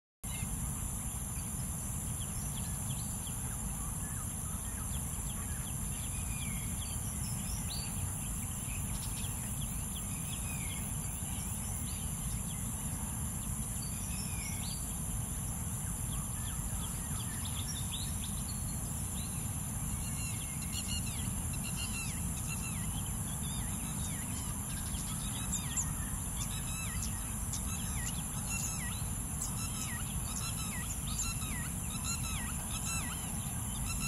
Greater Coucal | Olympus M.ZUIKO DIGITAL ED 150-400mm F4.5 TC1.25x IS PRO.
Greater Coucal sighted at Old Holland Road. This is an uncommon bird in Singapore. Camera is Olympus EM1 Mark 3 and lens is Olympus M.ZUIKO DIGITAL ED 150-400mm F4.5 TC1.25x IS PRO. Handheld.